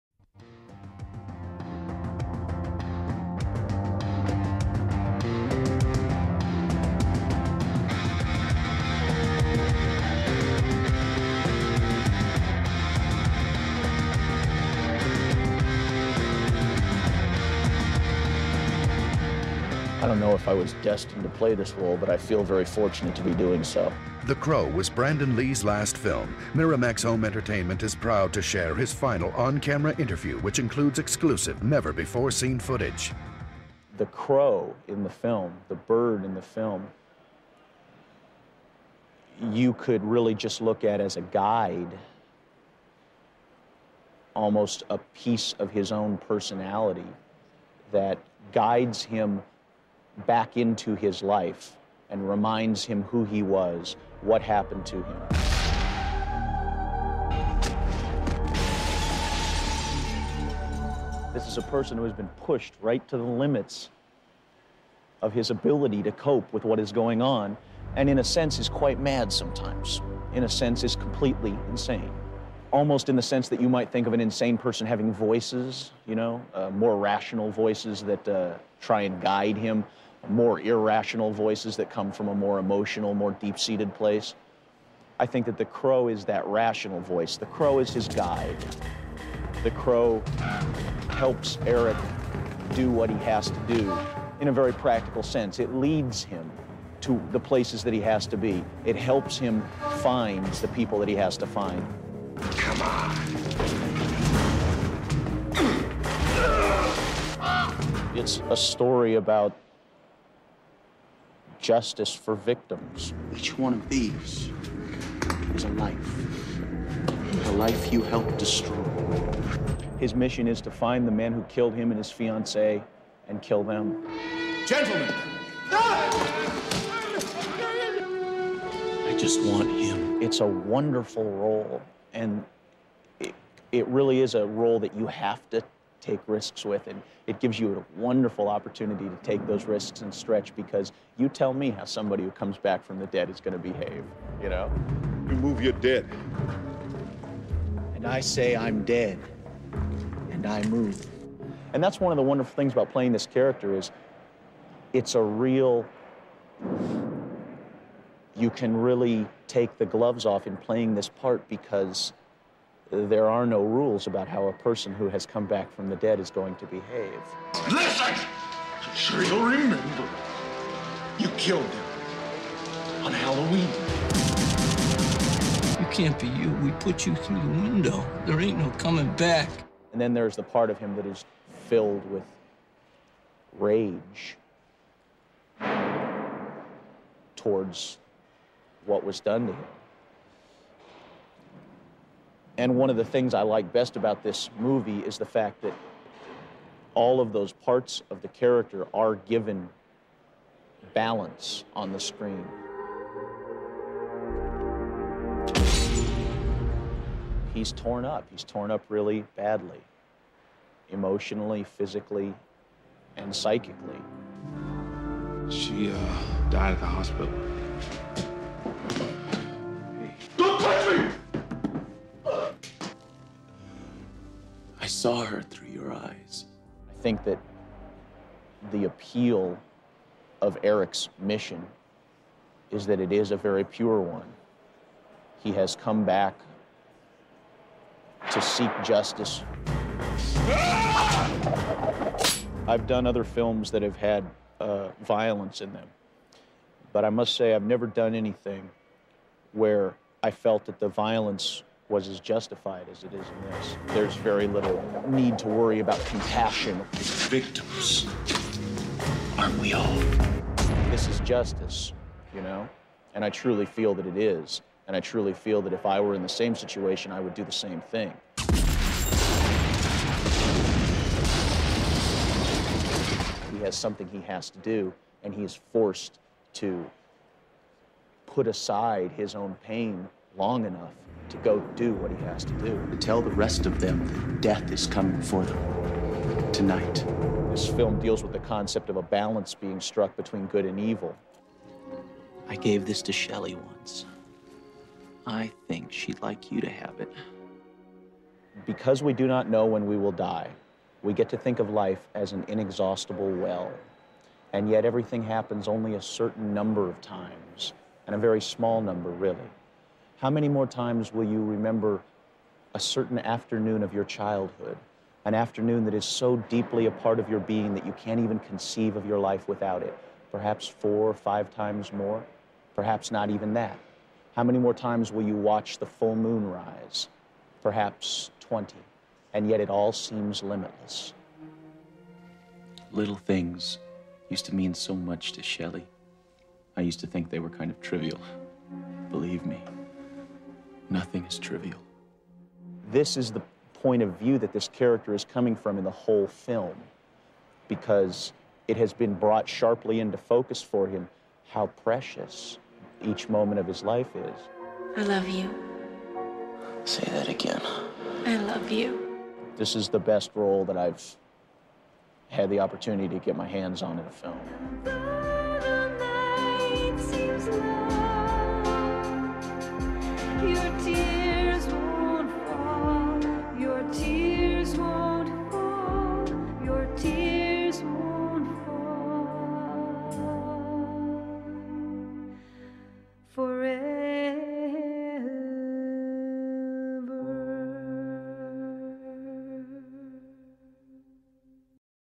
VIDEO: The Crow Branden Lee final interview
The final interview with Brandon Lee before his mysterious death on the set of The Crow. Chilling...